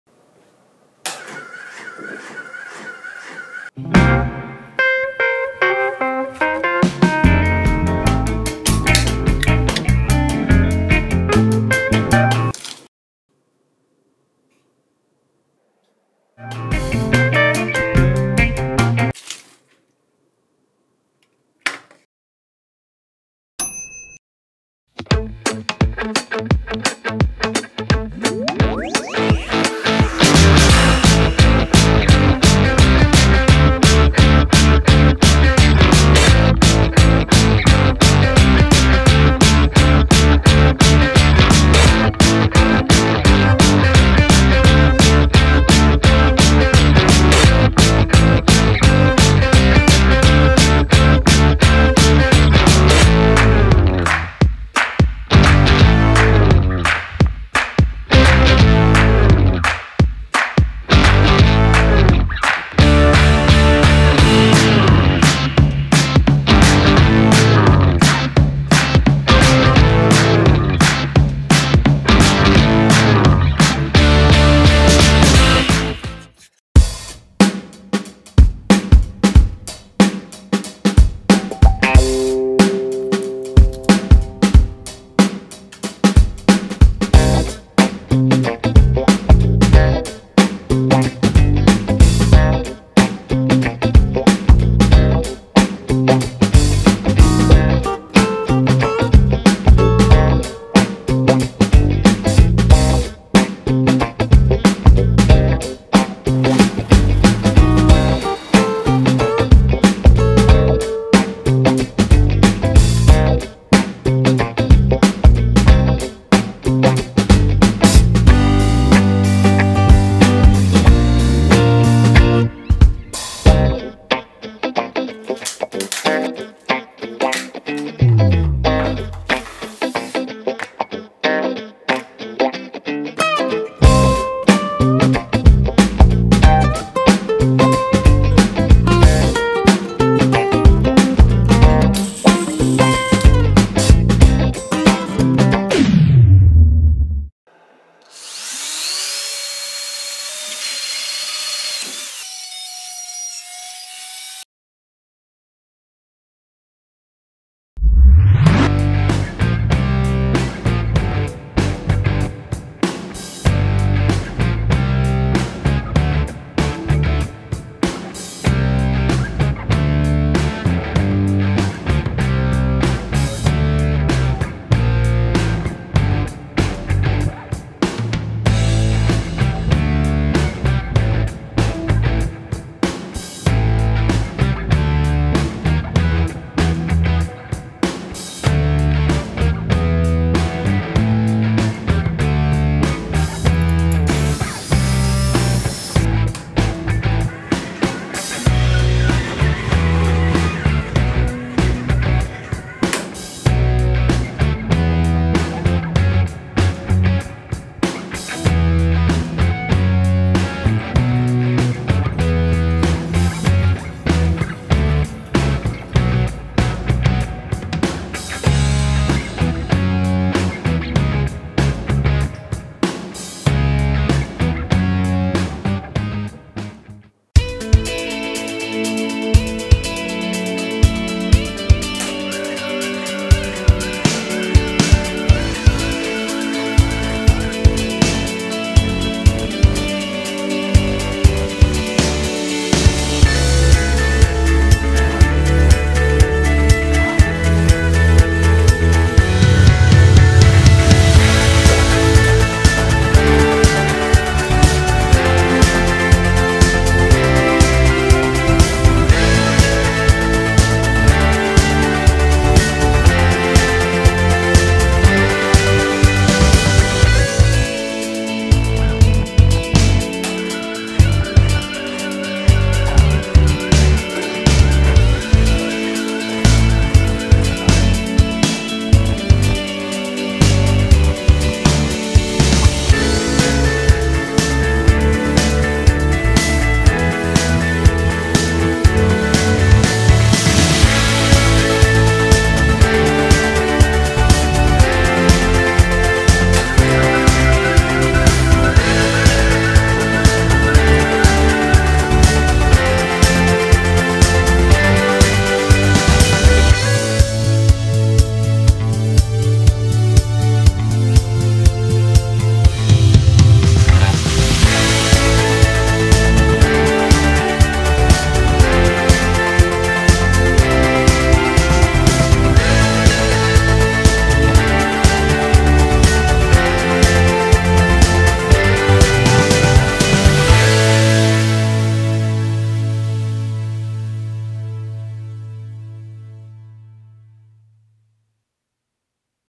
DIY Wave Tank - Physics of Tsunami Wave
In today's video I want to show you how to build wave tank that helps you to test different types of sea walls. It could be great science project with visualisation of waves behavior. Also wave tank could be used to simulate storm for test different ship types.